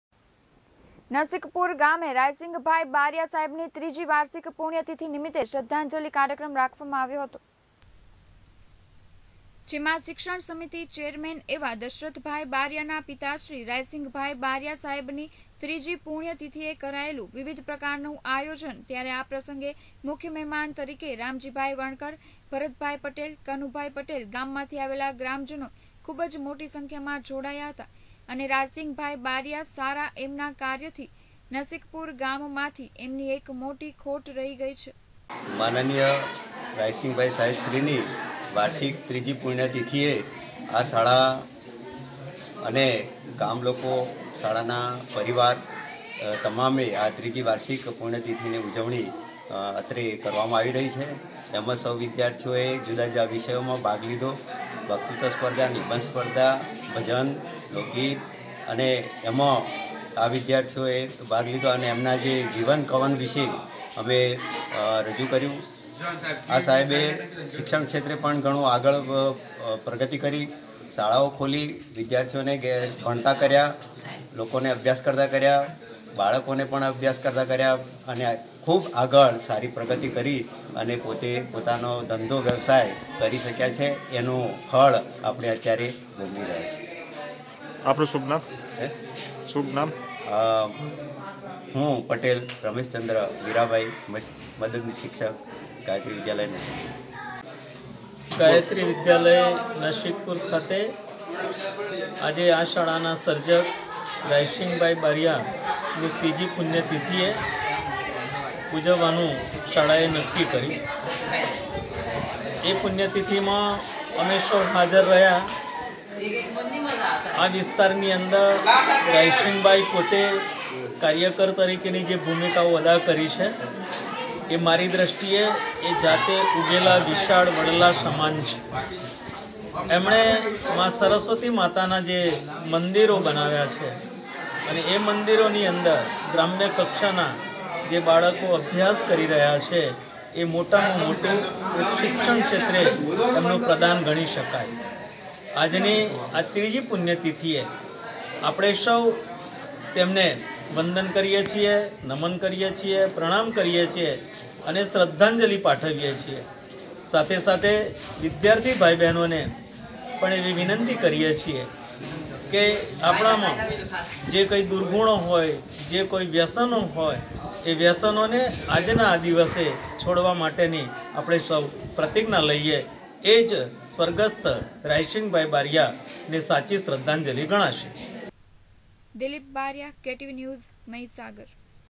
NASIKPUR: નસીકપુર ગામે રાયસીંગભાઇ બારીઆ સાહેબની ત્રીજી વાર્ષિક પુણ્ય તિથિ નિમિત્તે
NASIKPUR: નસીકપુર ગામે રાયસીંગભાઇ બારીઆ સાહેબની ત્રીજી વાર્ષિક પુણ્ય તિથિ નિમિત્તે શ્રધ્ધાંજલી કાર્યક્રમ યોજવામાં આવ્યો હતો.